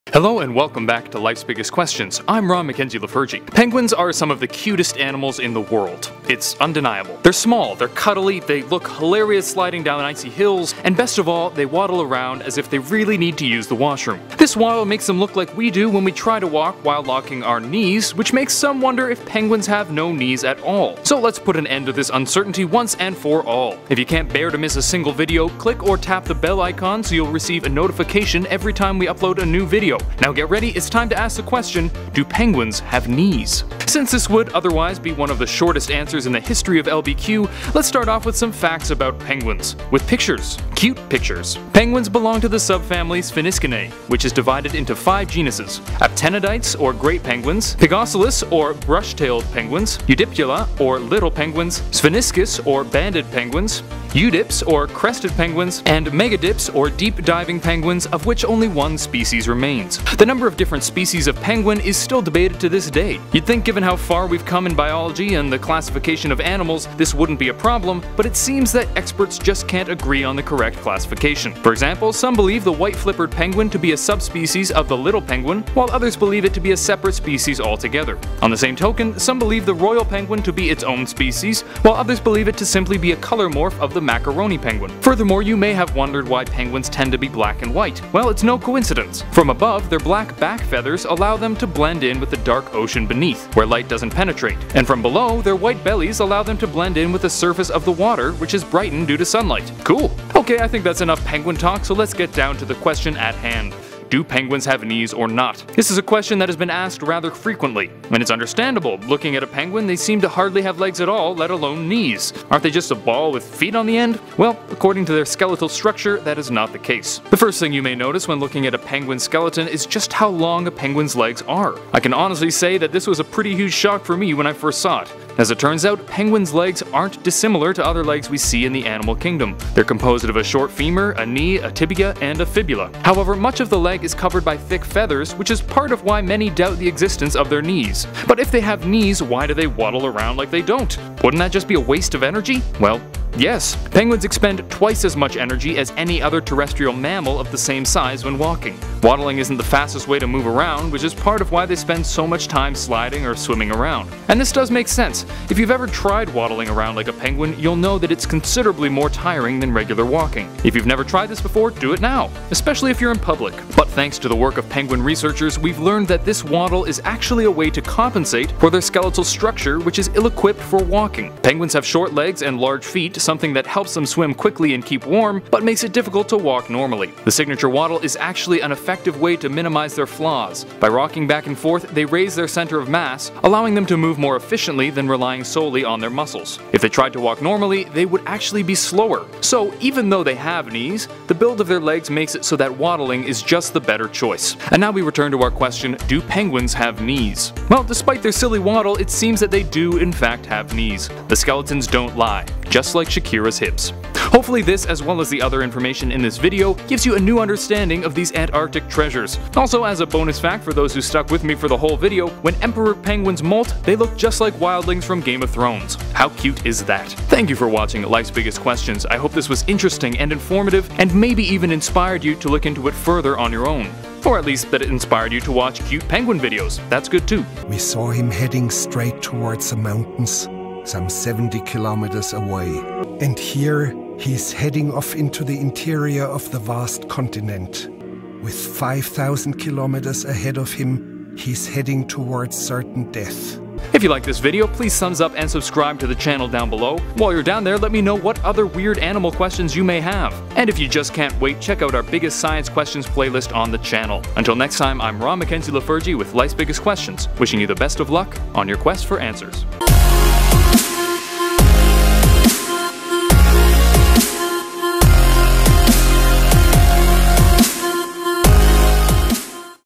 Do Penguins Have Knees?
Hello and welcome back to Life’s Biggest Questions, I’m Ron McKenzie-Lefurgey. Penguins are some of the cutest animals in the world. It’s undeniable. They’re small, they’re cuddly, they look hilarious sliding down icy hills, and best of all, the waddle around as if they really need to use the washroom. This waddle makes them look like we do when we try to walk while locking our knees, which makes some wonder if penguins have no knees at all. So let’s put an end to this uncertainty once and for all! Now get ready, it’s time to ask the question: Do Penguins have knees?

VIDEO EDITED BY:
Ryan Wazonek